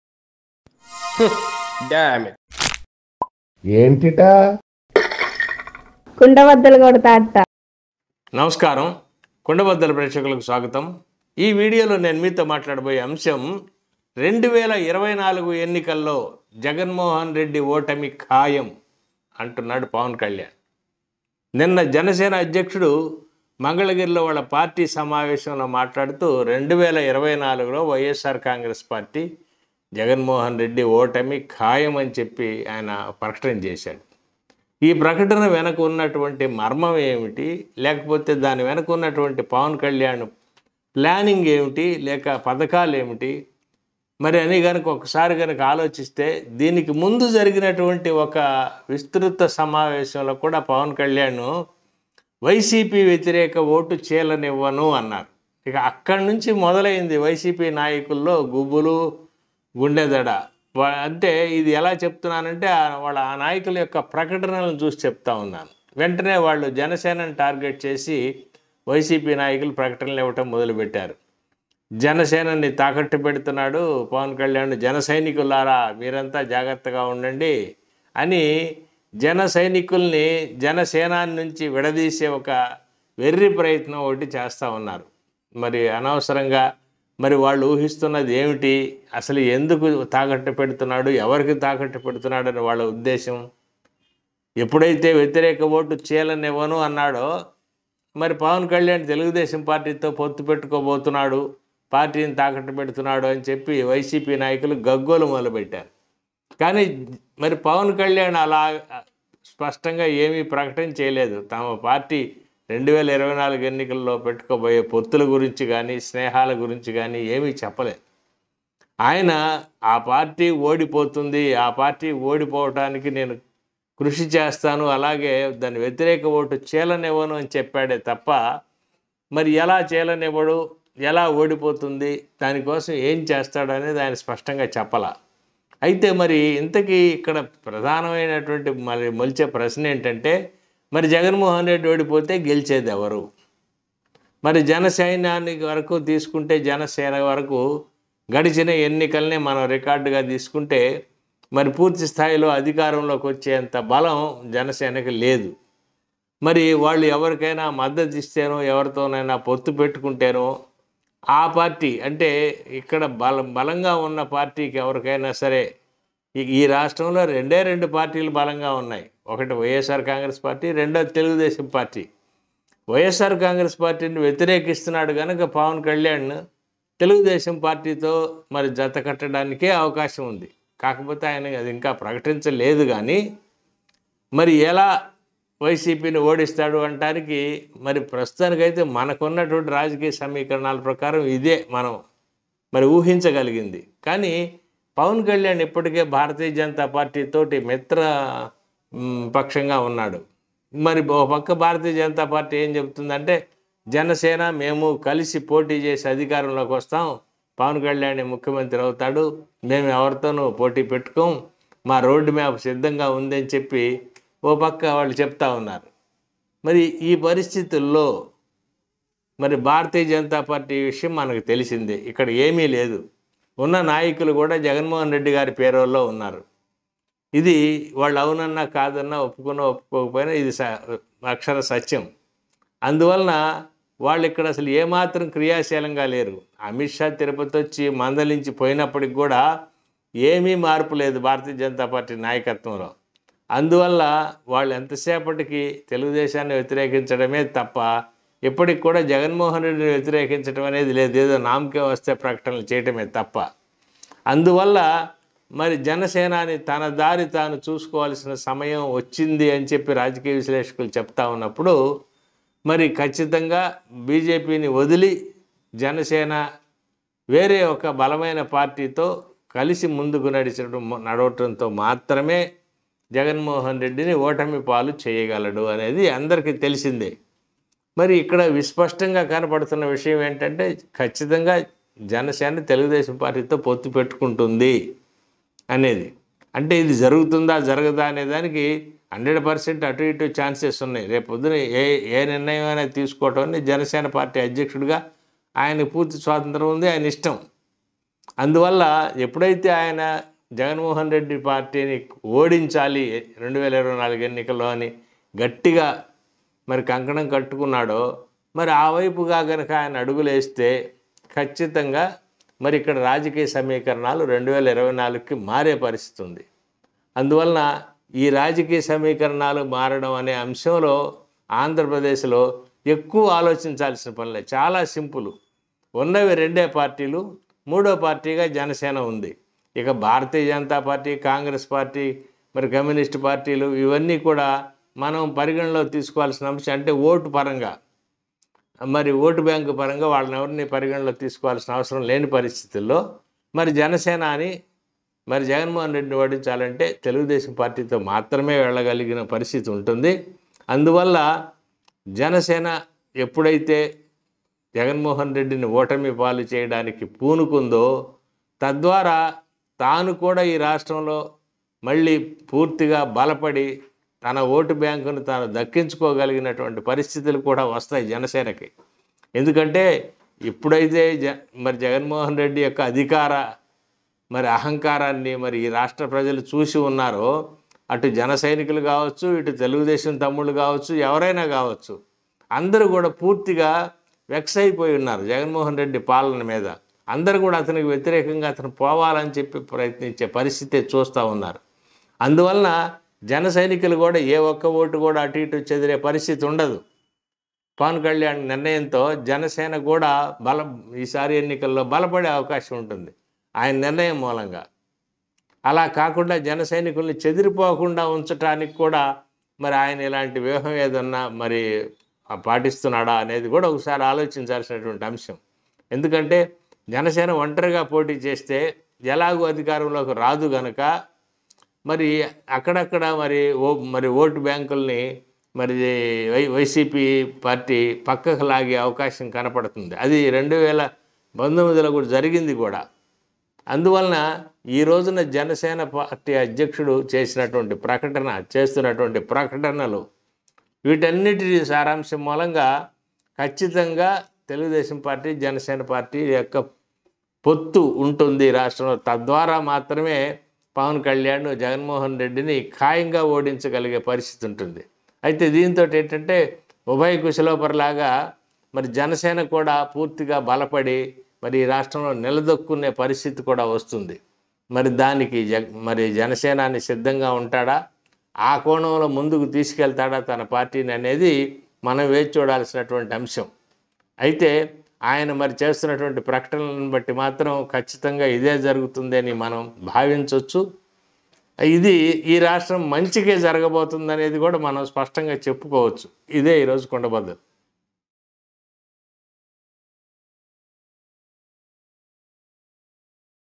2024లో YS జగన్ ఓడిపోతాడు అనే పవన్ వ్యాఖ్యల వెనకున్న మర్మం ఏమిటి? | Janasena Pawan Kalyan vs YS Jagan
AP CM YSRCP Party Leader YS Jagan Mohan Reedy,
Janasena Party Leader Pawan Kalyan,
Ambati Rambabu,
TDP Party Leader Chandrababu Naidu,
Kundabaddalu Subbarao,